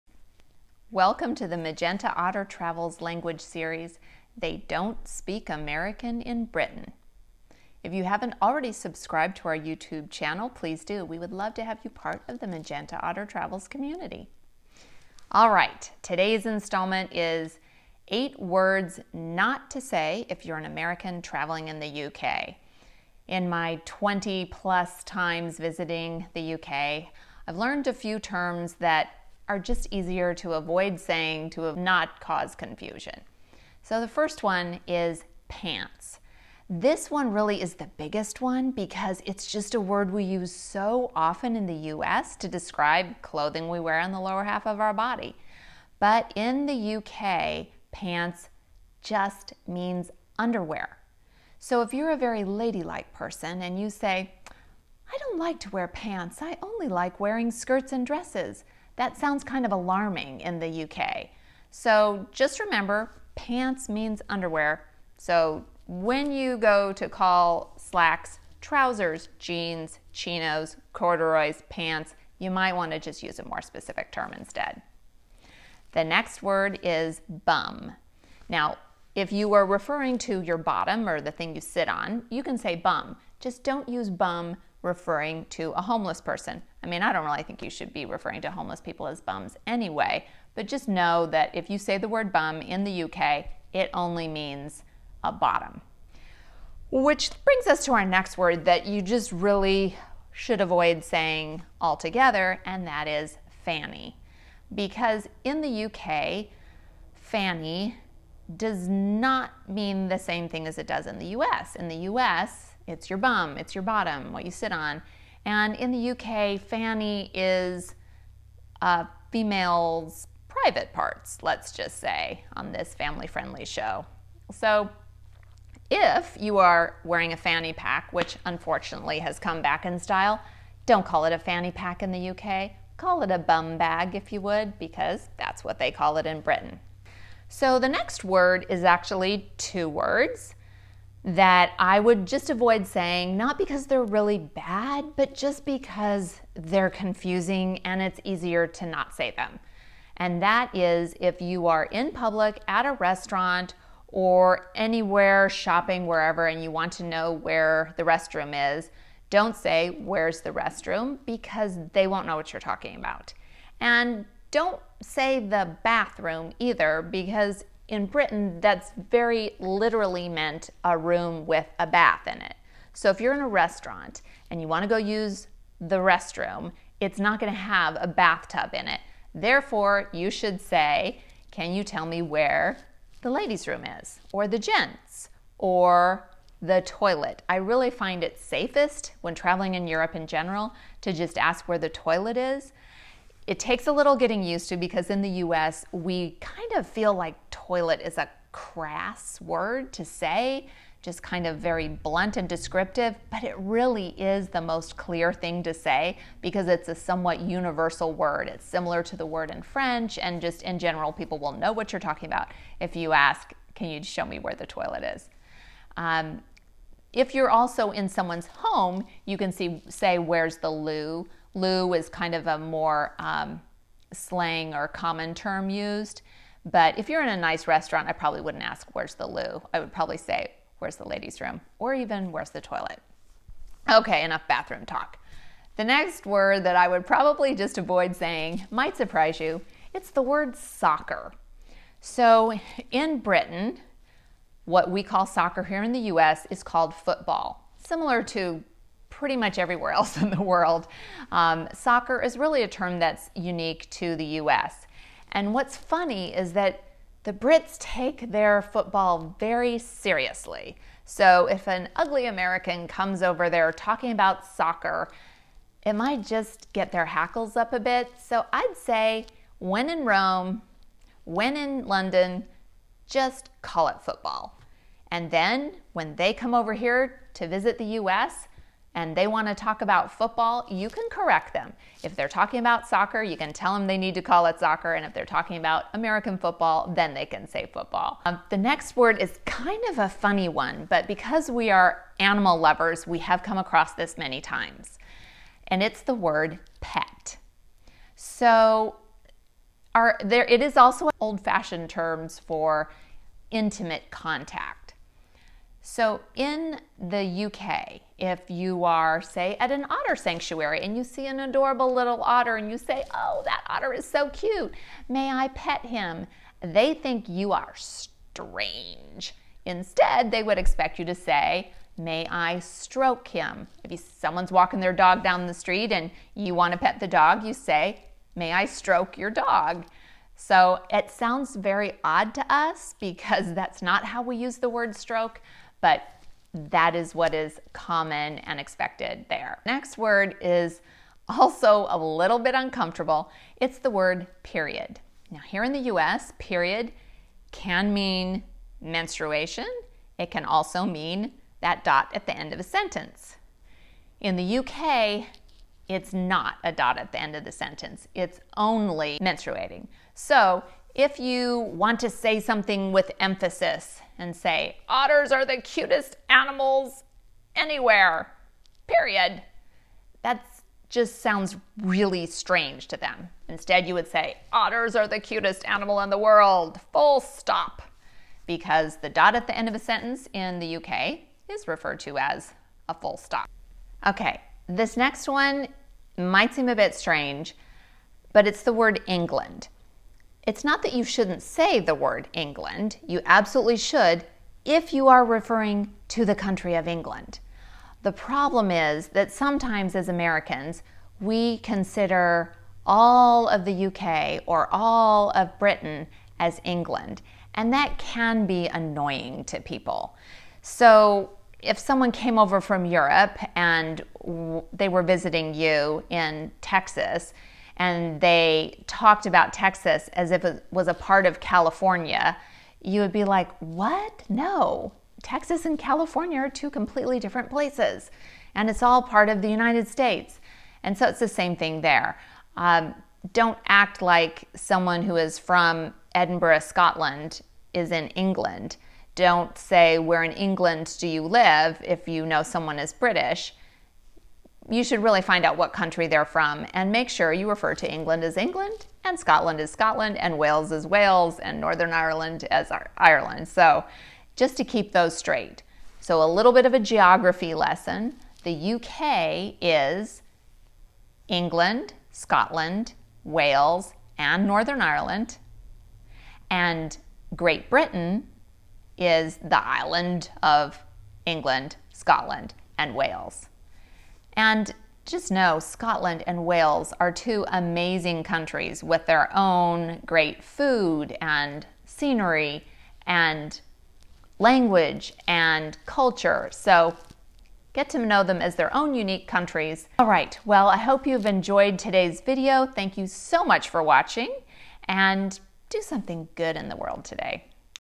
8 Words NOT to Say when Traveling in the England (American Traveling in England)
Here are 8 words you should avoid saying when traveling in the United Kingdom or Great Britain.  They either have different meanings in the US vs. the UK or they are just confusing or otherwise troublesome.  Instead, try these alternatives for more clear communication when visiting Britain!